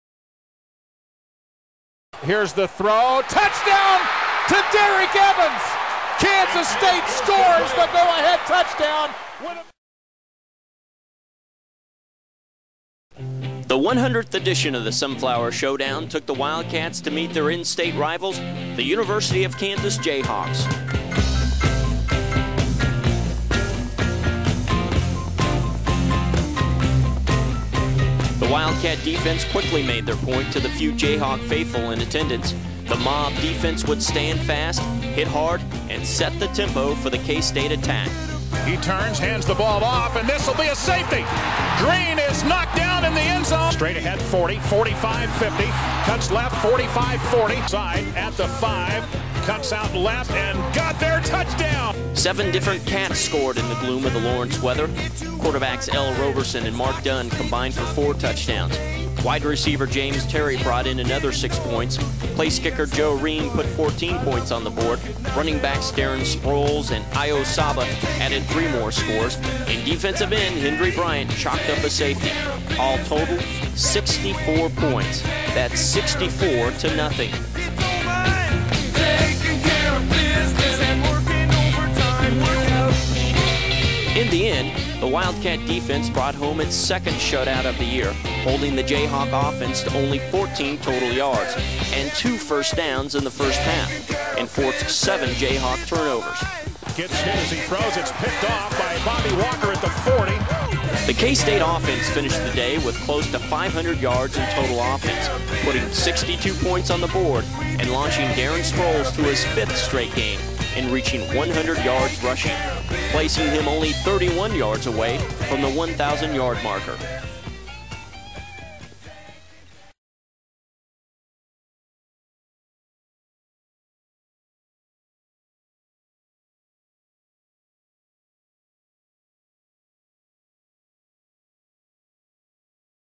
2002 KSU beats KU 64-0
KSU utterly dominates KU in Lawrence in a game witnessed by 27 KU fans.